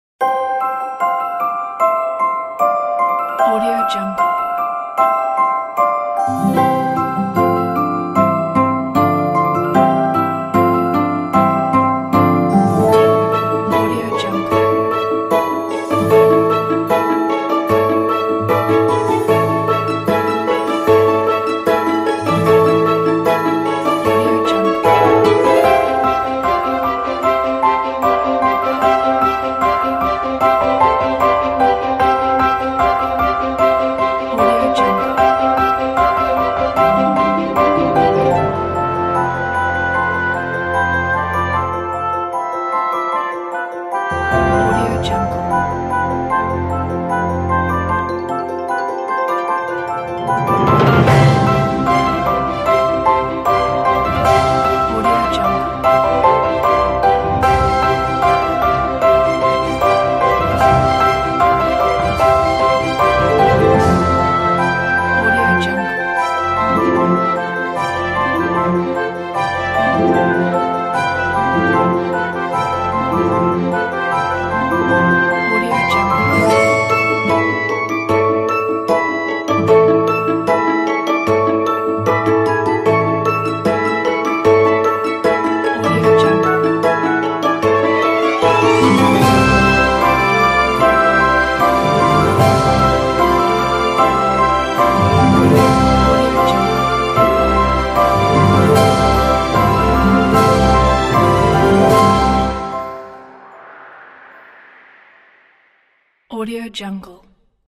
Christmas Gallery
Stylish christmas gallery with more that 30 hand drawn design elements. Use up to 38 photo / video placeholders to display your christmas memories to your family and friends. Includes short version with 12 placeholders and also can be used as a corporate christmas greeting or opener.
1080p, 720p
- Background color control
- Just drag and drop your photos or videos
- Long version: 38 images / videos (1min 42 sec)
- Short version: 12 images / videos (42 sec)
- No plugins required
- Optical Flares version included
- After Effects CS6 or higher
- CS5.5 version included
- 30 fps